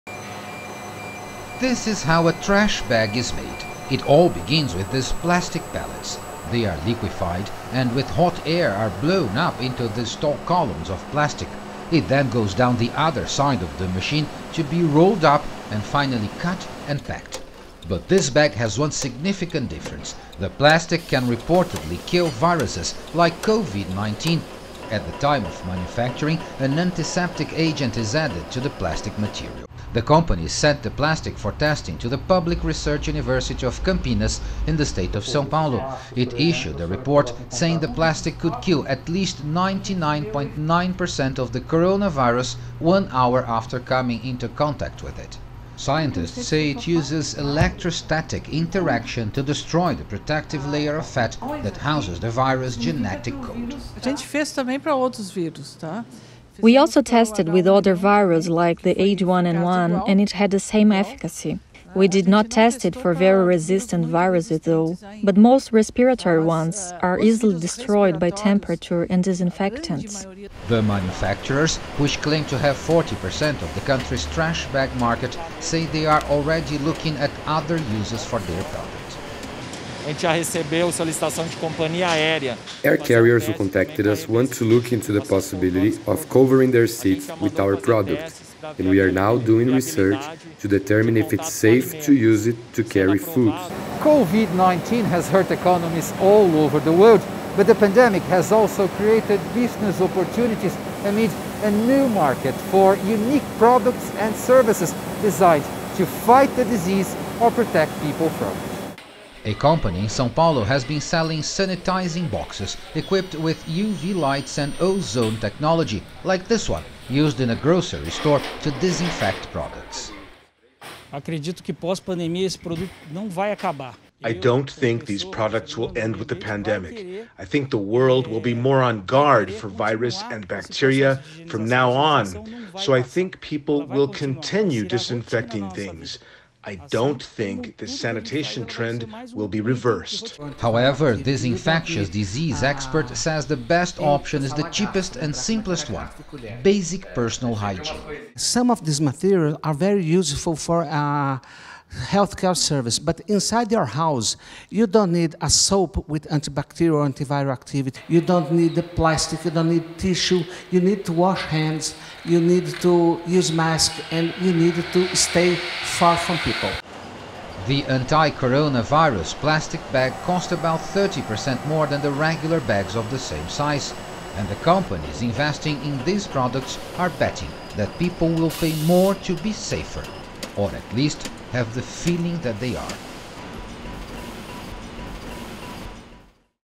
COVID-19: Can plastic help protect us?
A Brazilian company that makes trash bags has developed a plastic that it says can kill the coronavirus. It’s an example of the ingenuity of some businesses to help protect people against COVID-19. CGTN’s Paulo Cabral has this report.